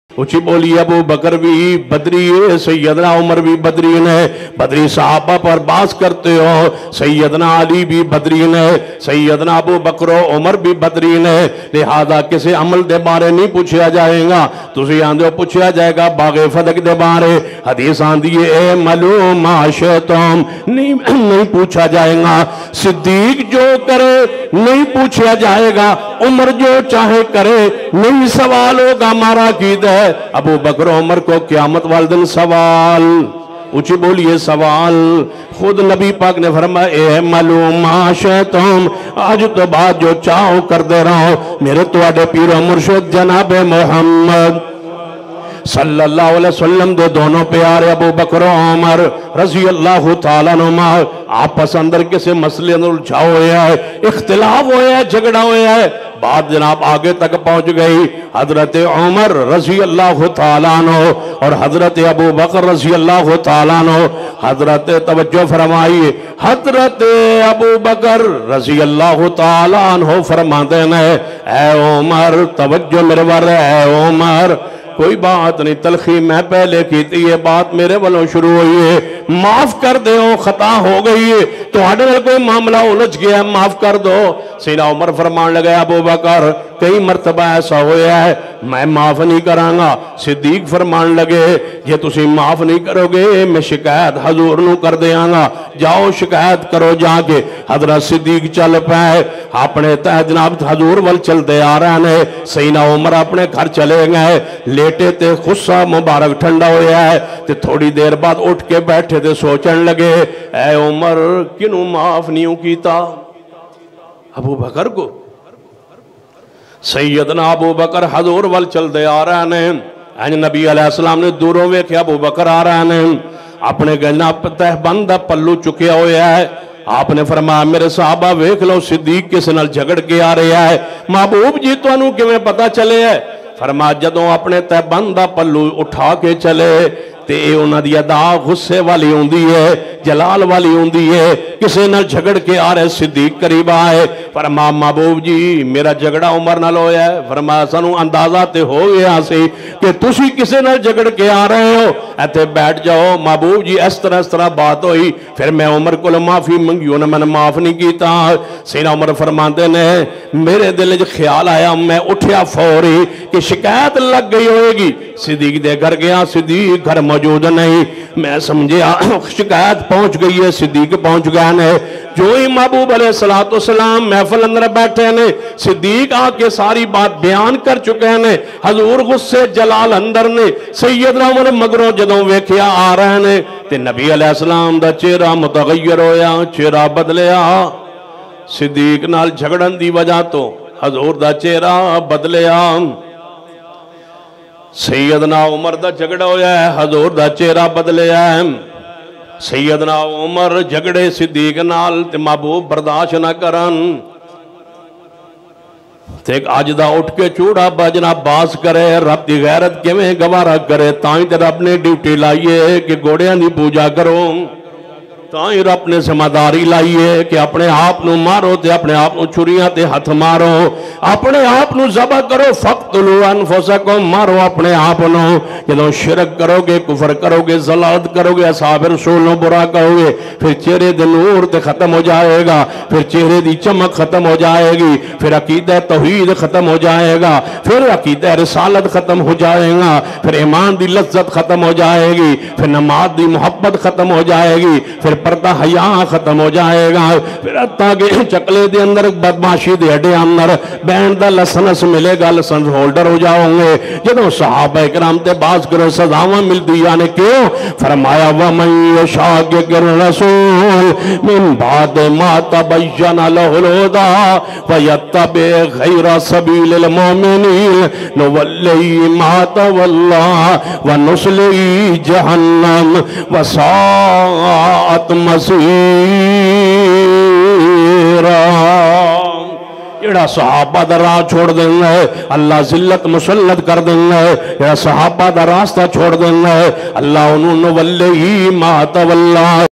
Shia Sy Munazra | Baghy Fidak Ka Zima Dar Kon? | Syed Sabtain Shah Naqvi 2023
کیا ابوبکر صدیقؓ باغِ فدک کے ذمہ دار ہیں؟
🎙️سیـــد سبطـین شـــاہ نقــوی حفظہ اللہ تعالیٰ

And Please Don't  Forget To Click The "SABSRAIBE" Button😍Jazakallah❤️